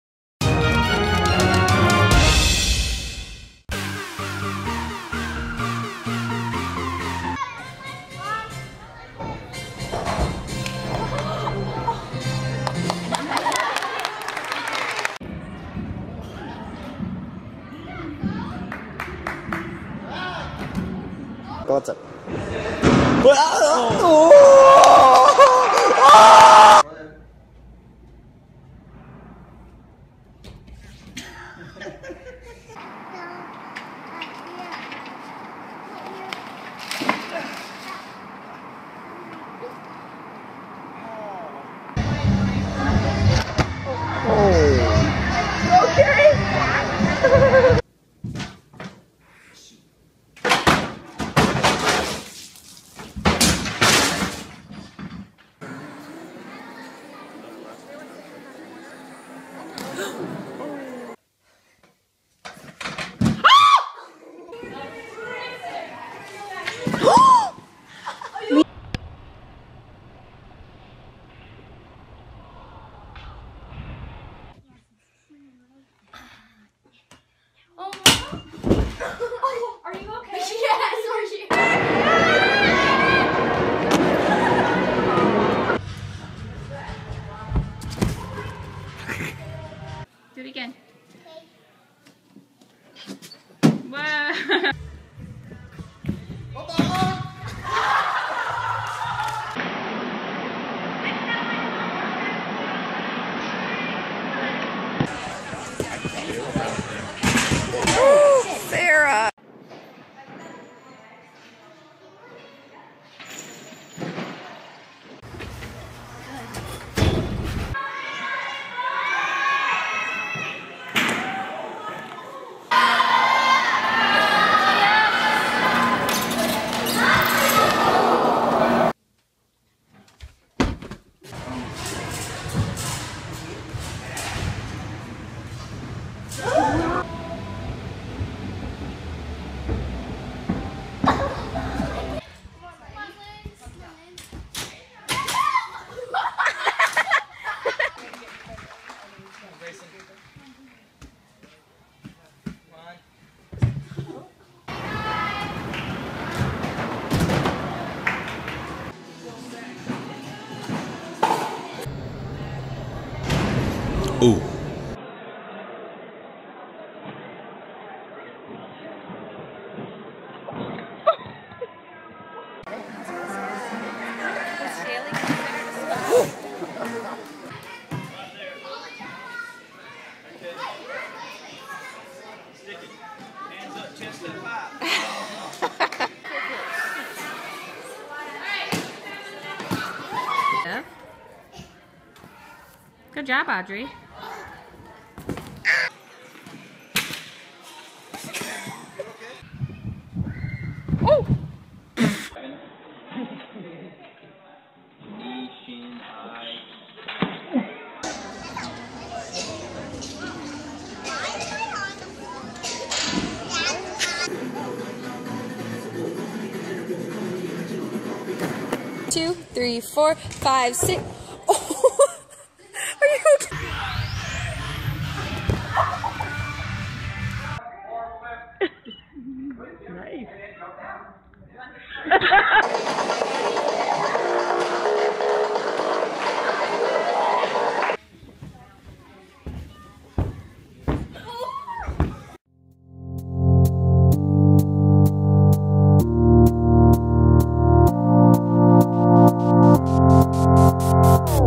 Funny GYMNASTICS fails | compilation
I wanted to make a funny video istead of a painfull one. So I tried to make this not too painfull!

catogoossens
gymnasticsshout_out

-A request
-Something sweet ♥